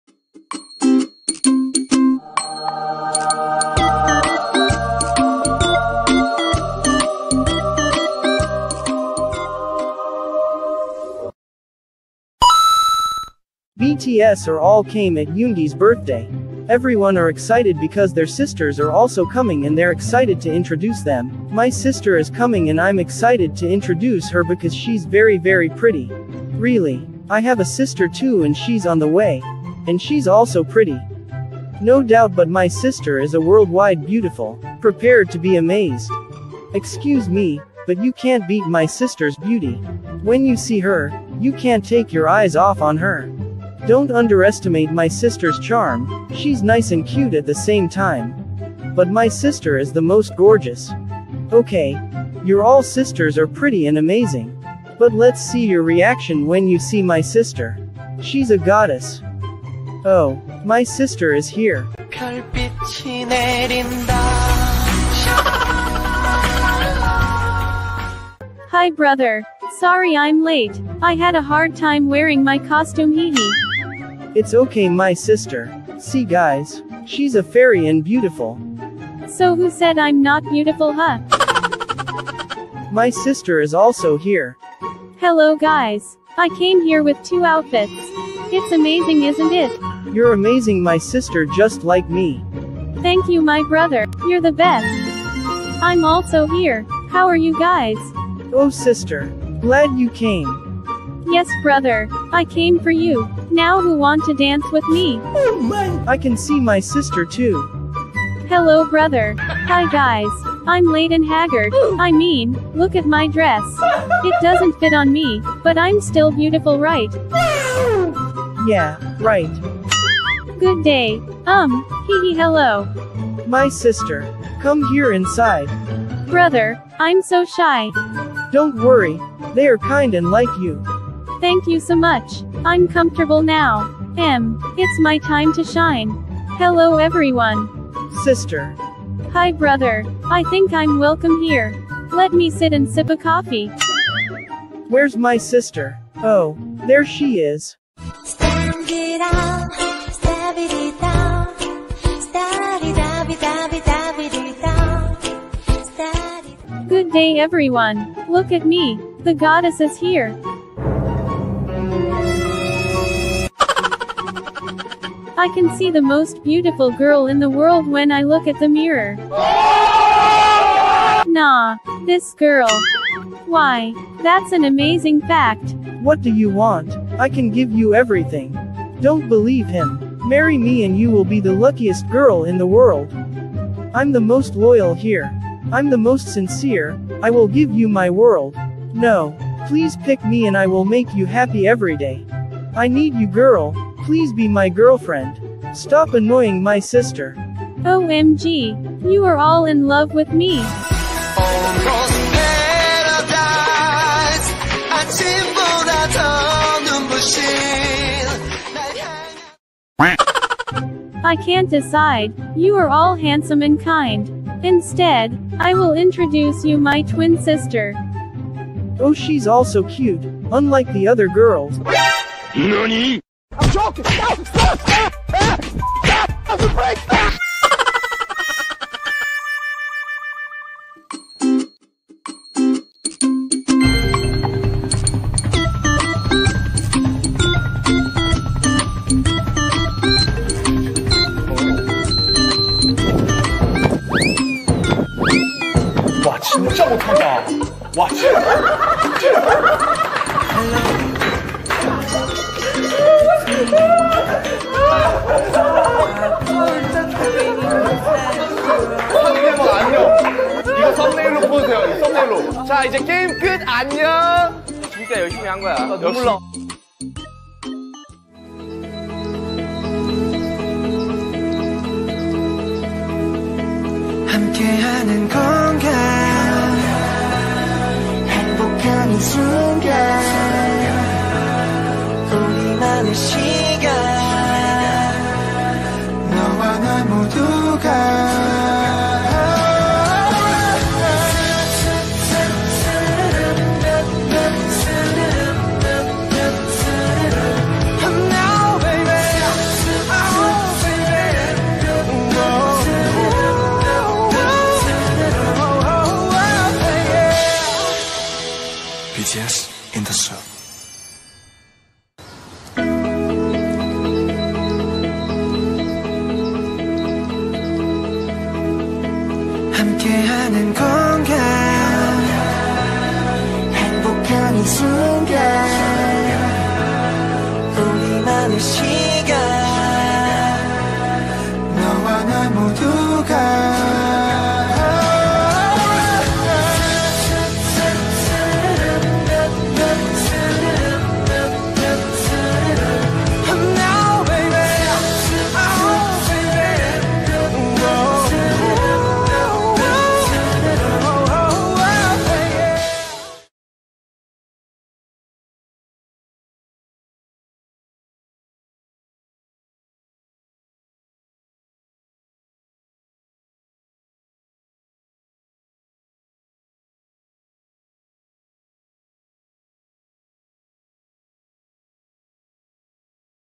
BTS and their Sisters 😂 (cut part) FMS
Their Sisters BTS (cut part) Fan made Skit

Scene 1 Jin is leaving
Scene 2 V's noodles is missing
Scene 3 BTS and their sisters

( Sorry if my English is not so good but I hope you understand.

BTS Fan Fiction Story Fan Made Skit
by Laissa Seokjin